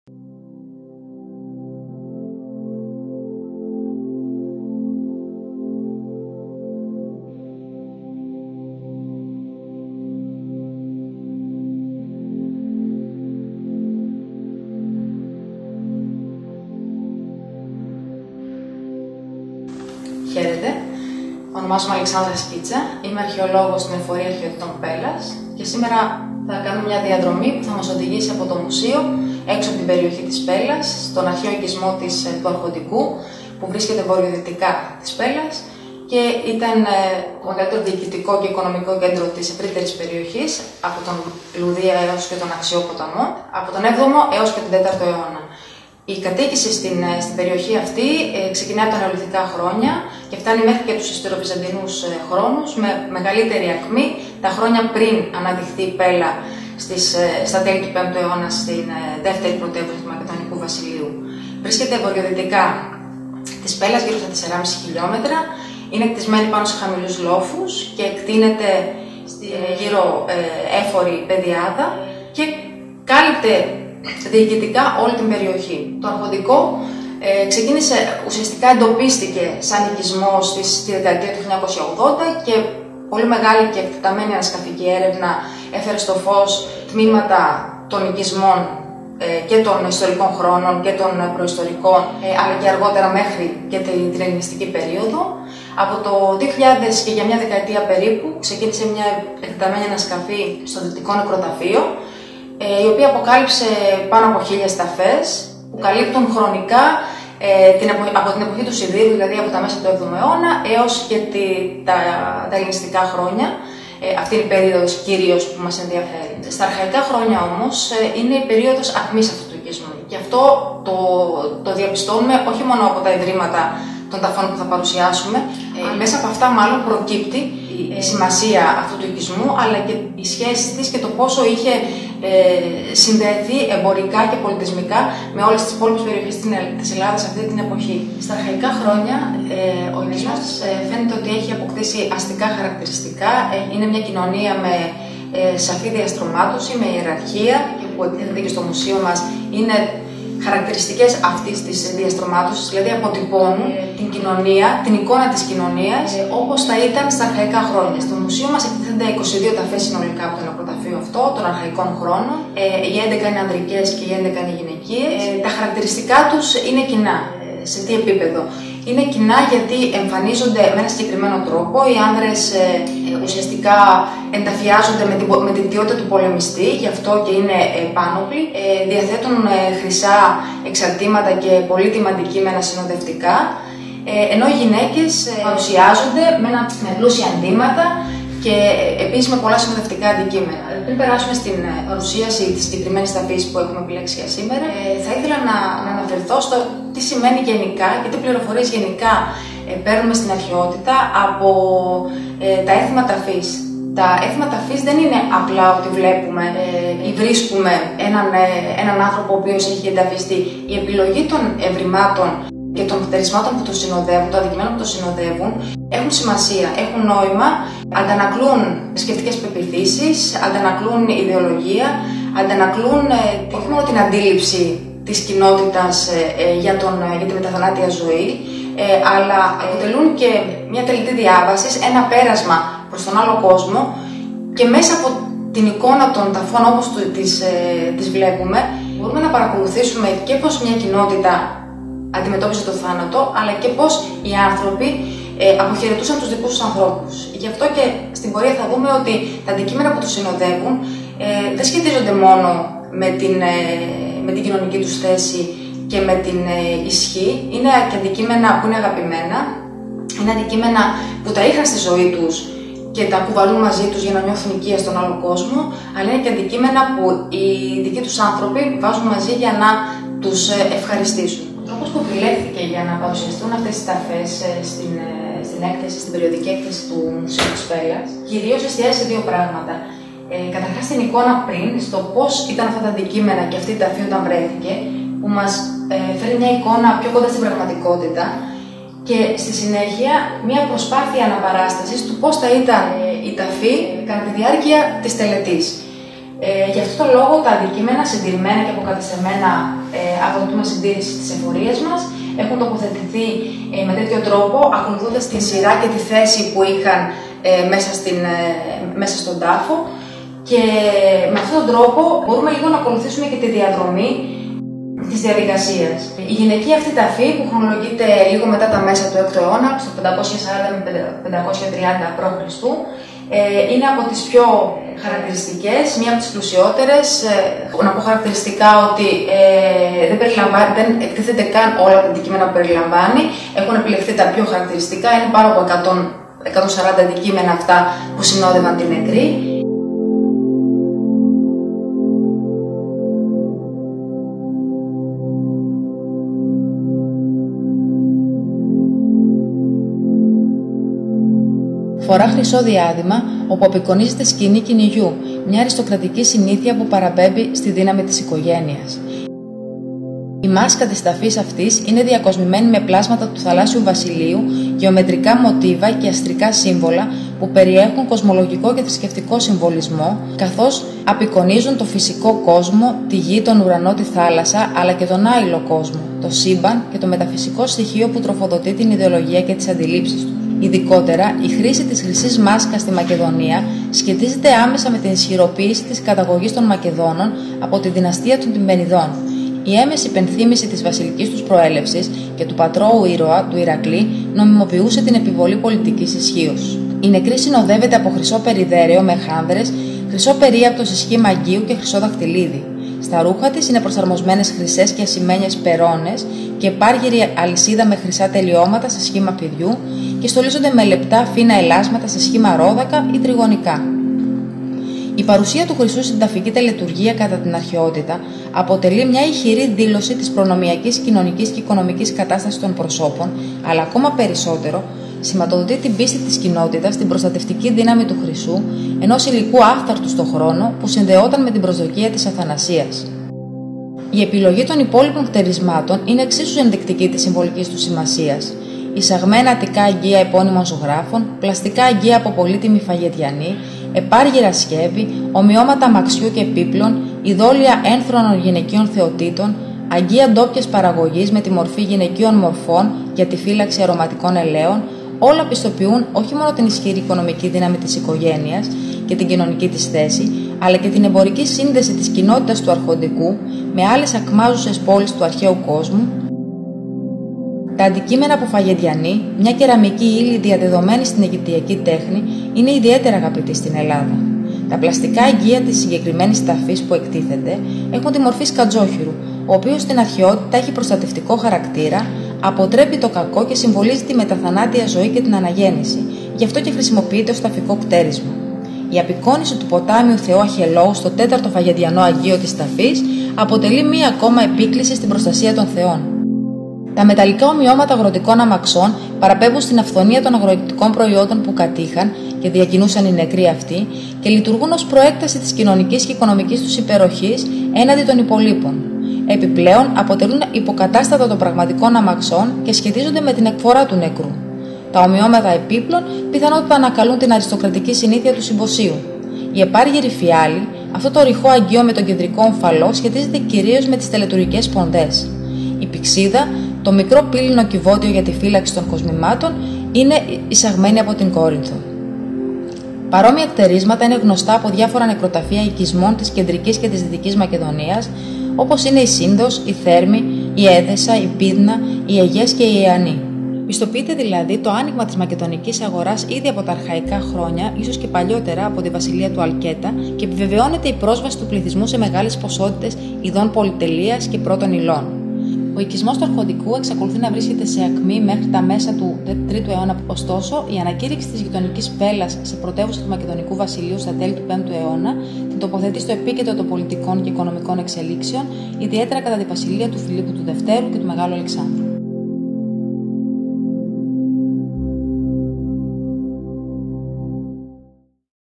Μια εξέχουσα κυρία από το Αρχοντικό Πέλλας
Το ''Μουσείο χτυπά την πόρτα σας''... ξανά και σας ταξιδεύει ψηφιακά στον αρχαίο οικισμό του Αρχοντικού Πέλλας, για να σας συστήσει μια εξέχουσα κυρία των αρχαϊκών χρόνων που θα  αποκαλύψει τα μυστικά της... μέσα από την παρουσίαση της αρχαιολόγου της Εφορείας Αρχαιοτήτων Πέλλας, Αλεξάνδρας Σκίτσα.